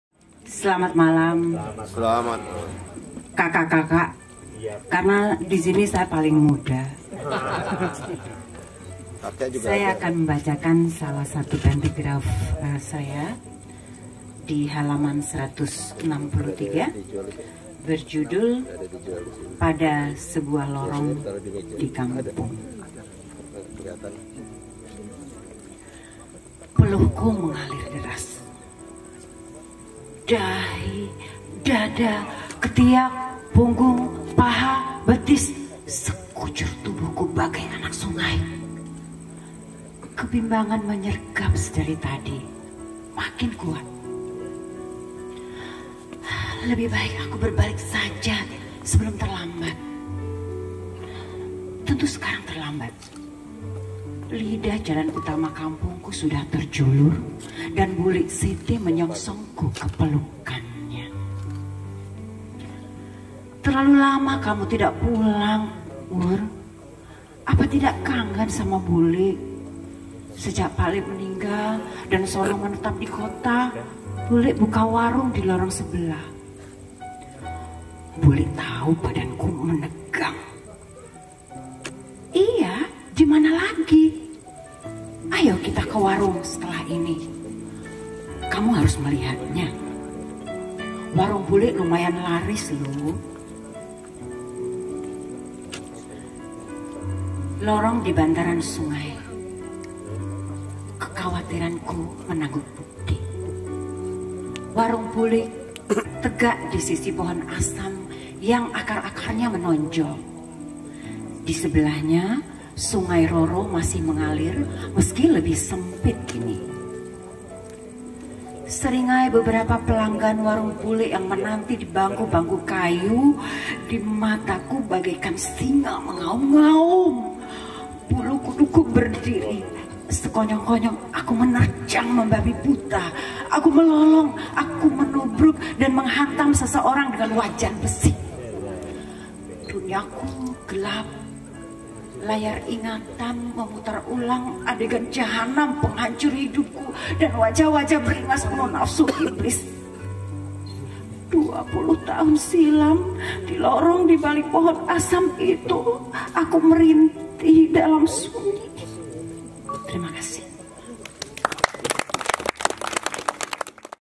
RINNY SOEGIYOHARTO
RINNY SOEGIYOHARTO membacakan karyanya yang termuat dalam Kitab Pentigraf 2 "Papan Iklan di Pintu Depan"

Dalam rangkaian acaara
SASTRA BULAN PURNAMA '83

Tembi Rumah Budaya Yogyakarta - 24 Agustus 2018